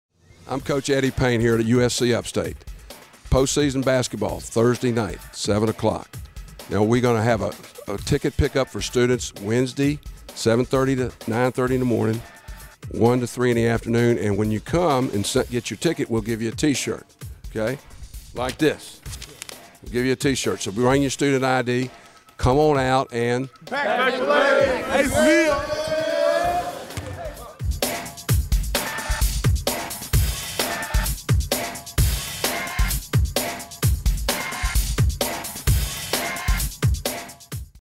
Upstate MBB: A Special Message
Upstate hosts Kent State Thursday March 15, at 7PM. Here is a special message from the Upstate Men's Basketball Team.